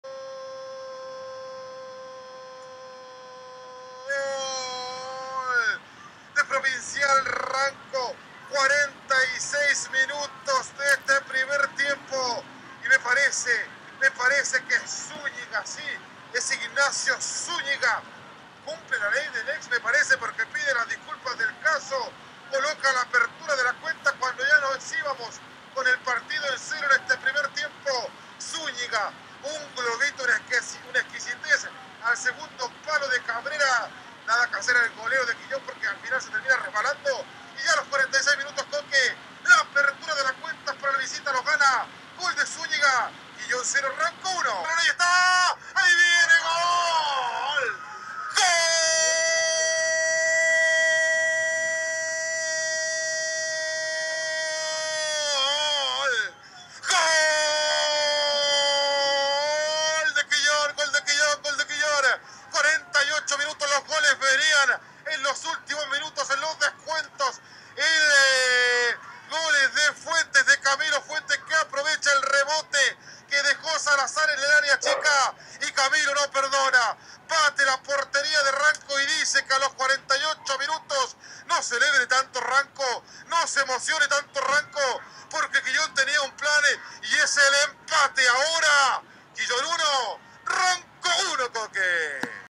RESUMEN: DEPORTES QUILLÓN 1 - 1 PROVINCIAL RANCO
Goles del partido entre Deportes Quillón y Provincial Ranco por la fecha 11 del Grupo Sur de Tercera A.